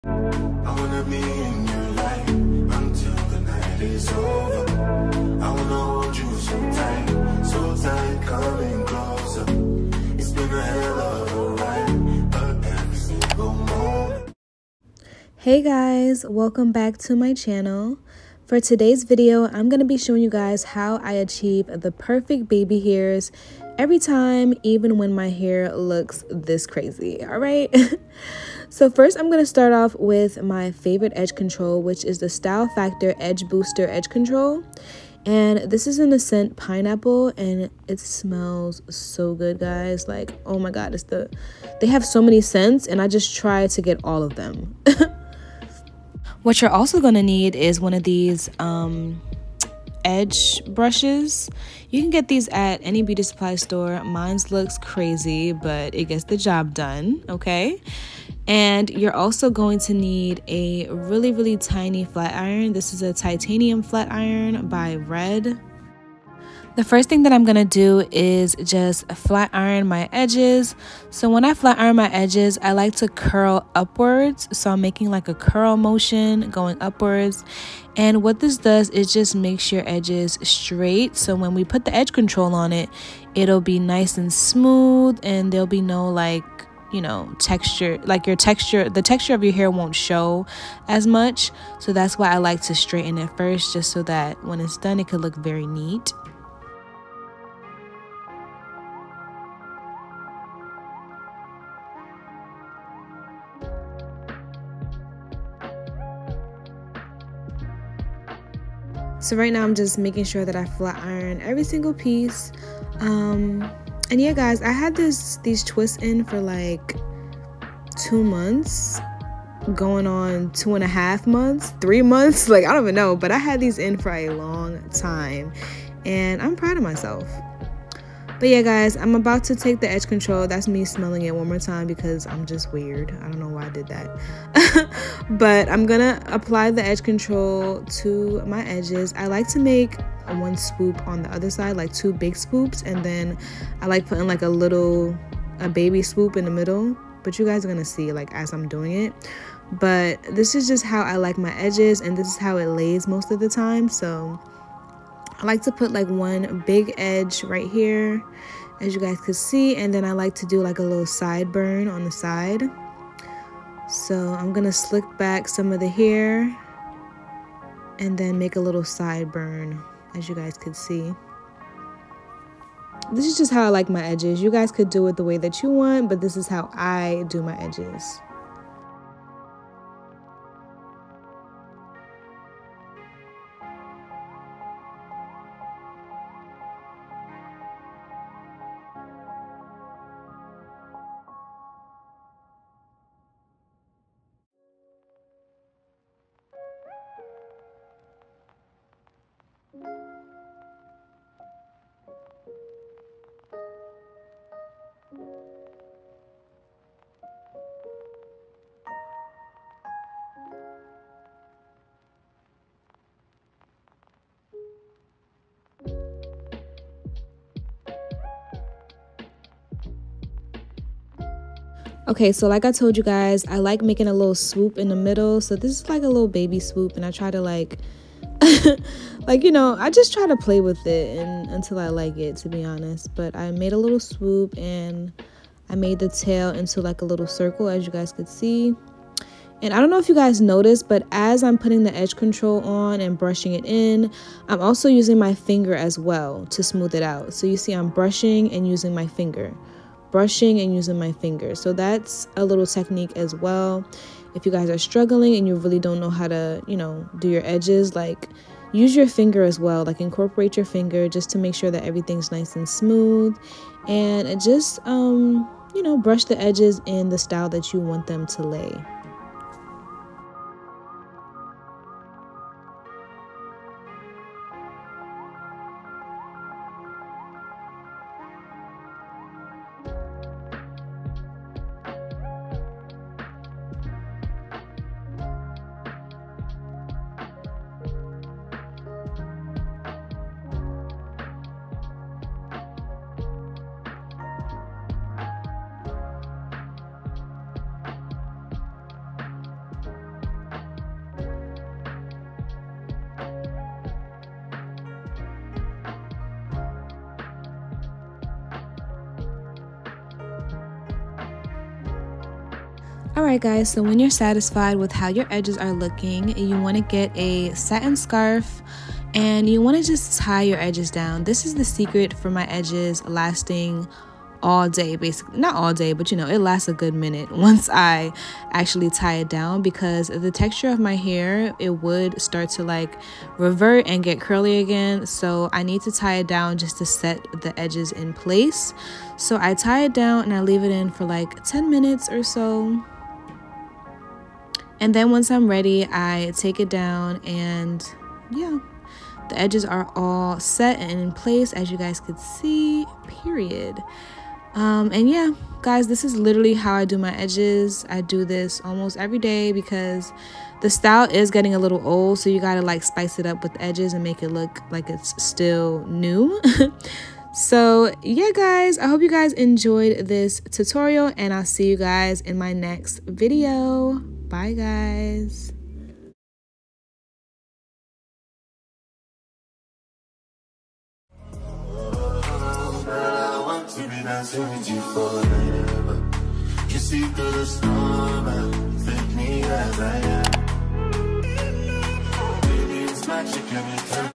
How to do perfect BABY HAIRS everytime!
Today I'm giving you details on how I do my baby hairs!

@JUICYBUNDLESS LASHES & hair - CODE: neva

♡EQUIPMENT:
Camera: Cannon & IPhone 14
Editing Software: iMovie
Lighting:  Ring Light | Neewer Ring Light 18" Amazon